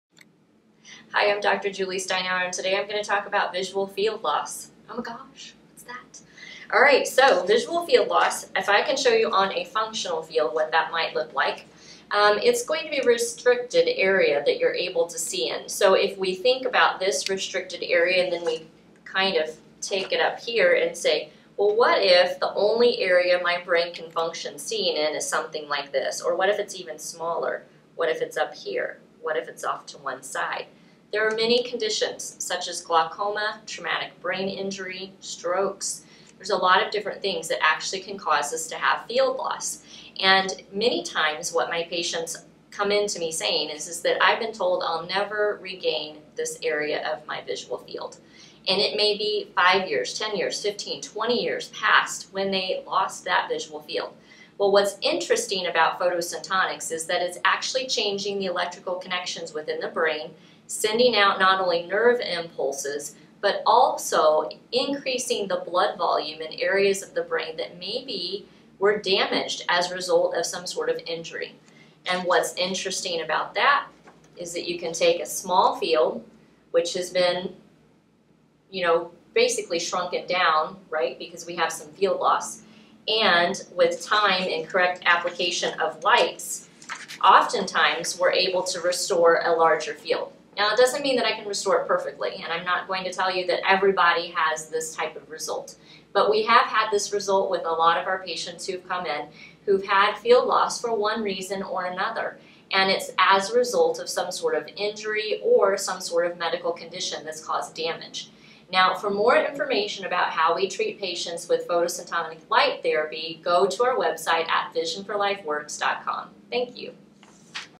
Visual Field Loss
Dr. Julie explains what this is, symptoms to look for, and how we treat it.